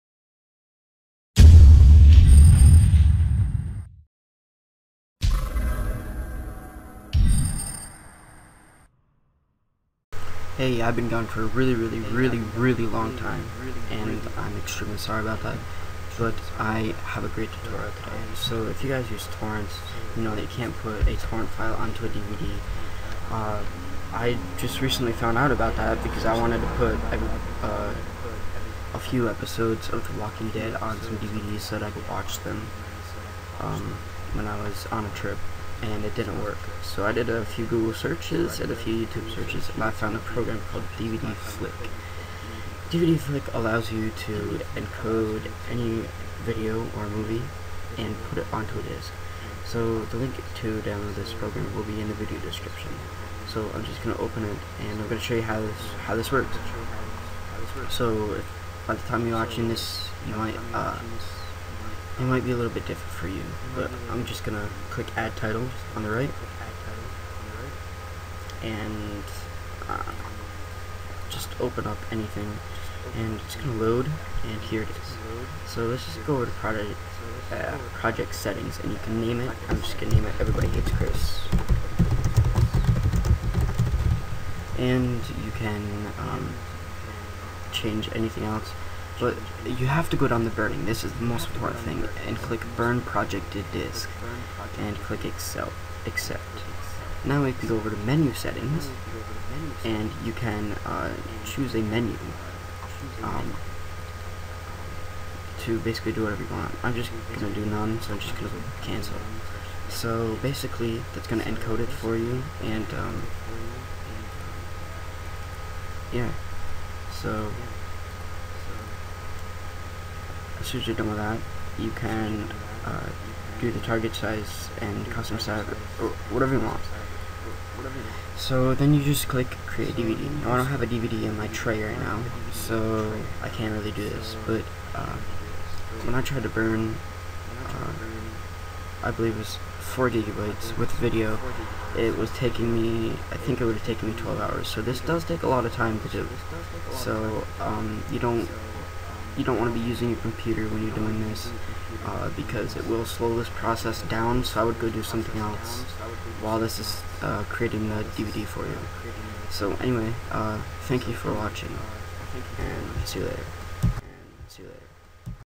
How to Burn Downloaded Movies Onto a DVD Disk
Very simple and easy tutorial on how to burn torrent movies onto a disk.